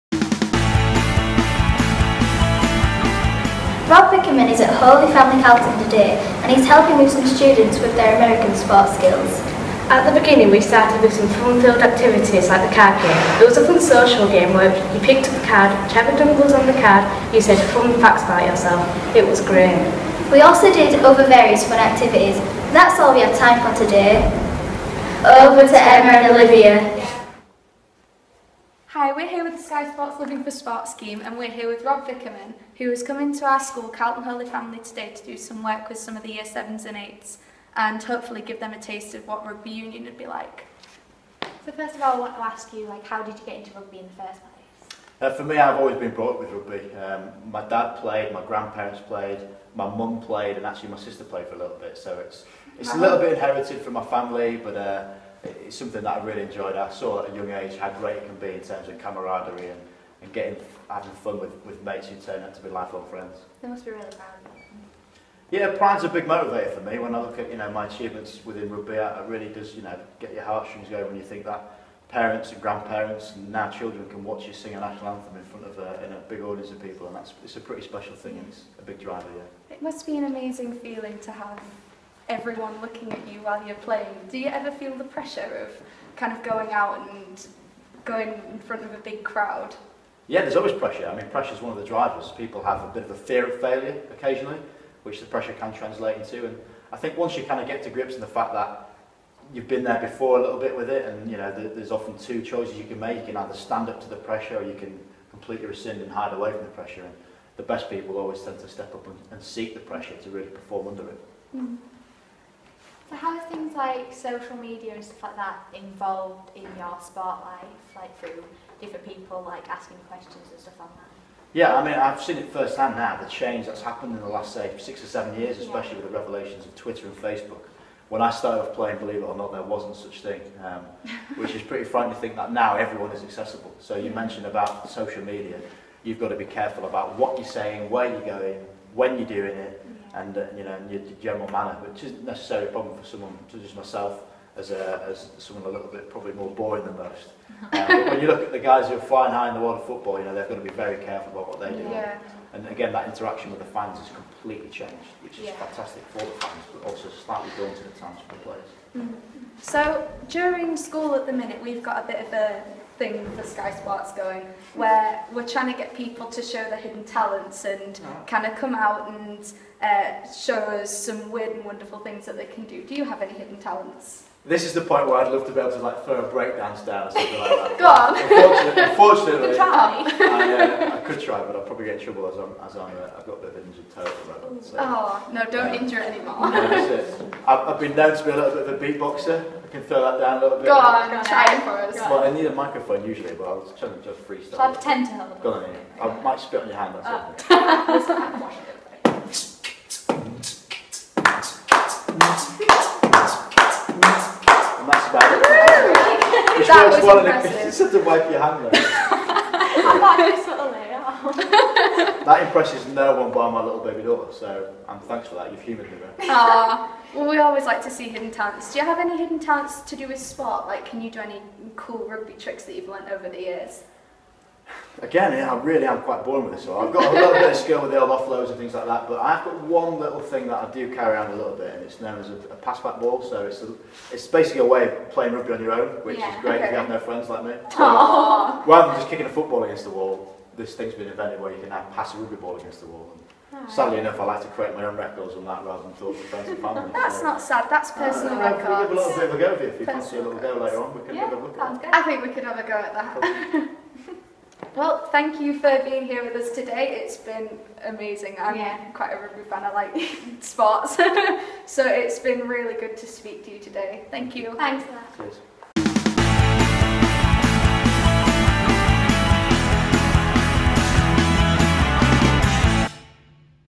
Sky Sports Living for Sport Launch Day Part 1
Interview with Rob Vickerman on launching Holy Family Carlton's Sky Sports Living for Sport project for 2014/2015 Part 2 includes video and photos from the day